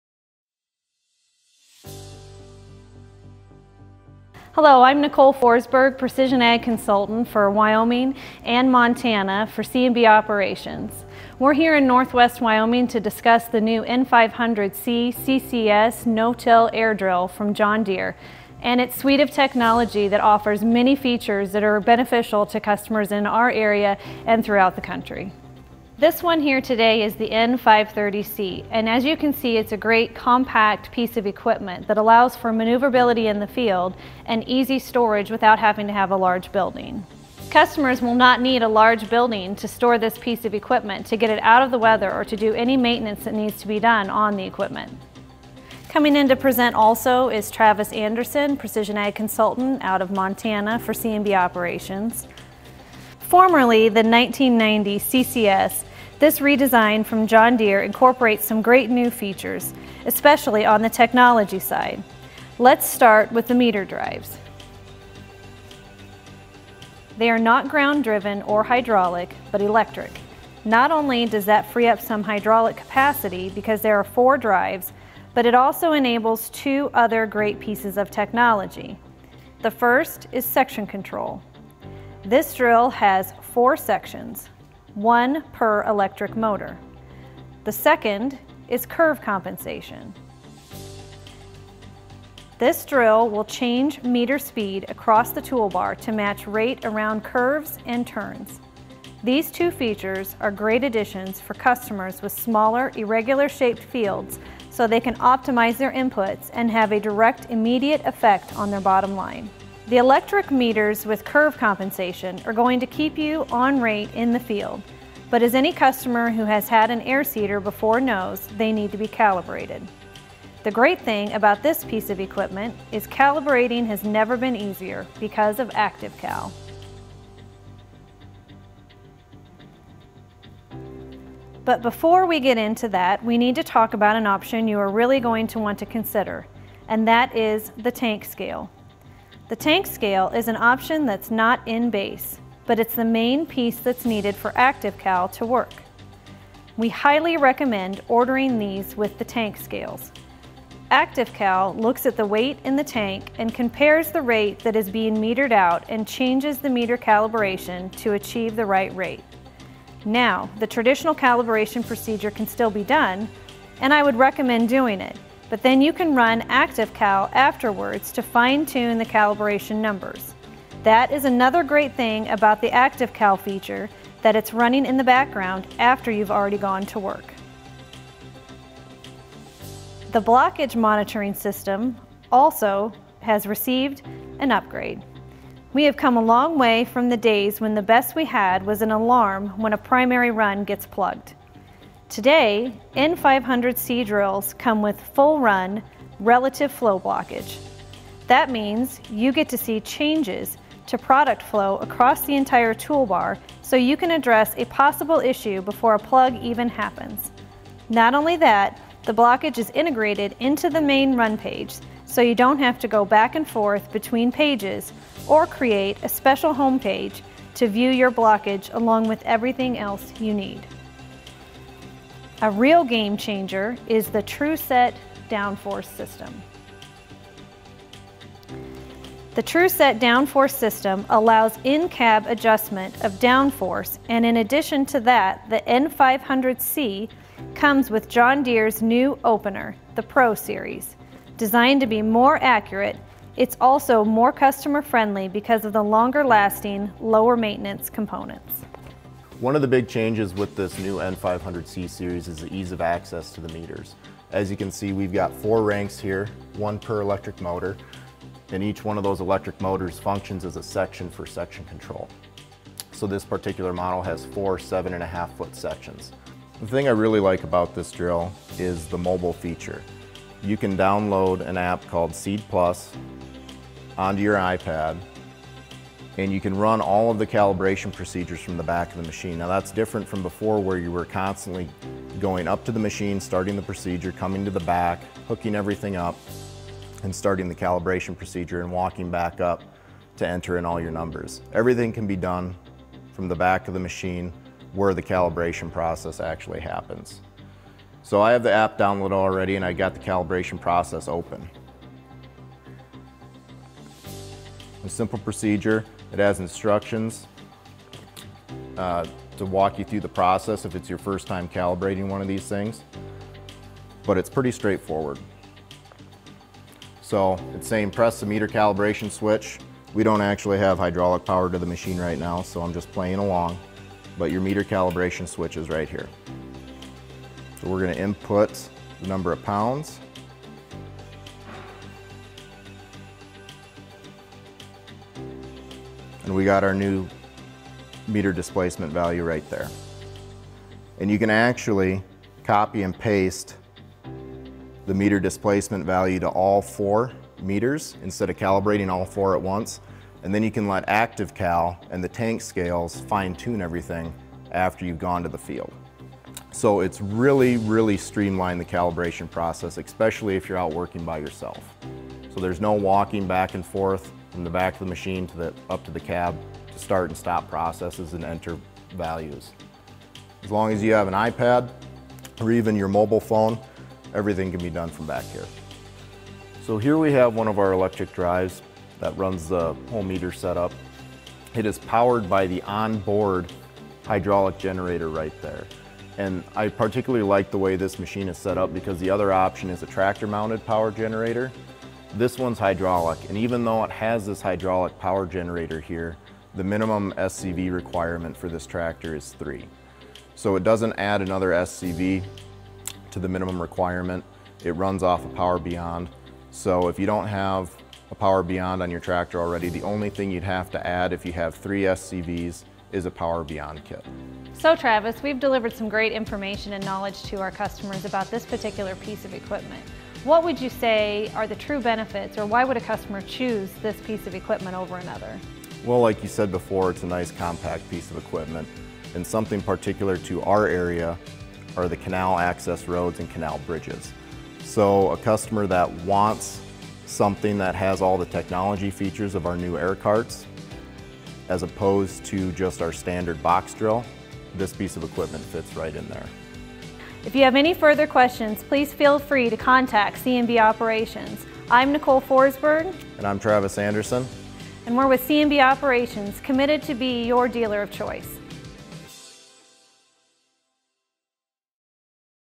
Virtual Tech Days 2021 - Day 1: John Deere N500C Air Carts
Nicole Forsberg and Travis Anderson, Precision Ag Consultants for C & B Operations, share an overview of the new John Deere N500C Air Cart.  They will walk through the N500C Air Car and show the improved features. Watch to also learn how to calibrate your drill using the Seed Plus App.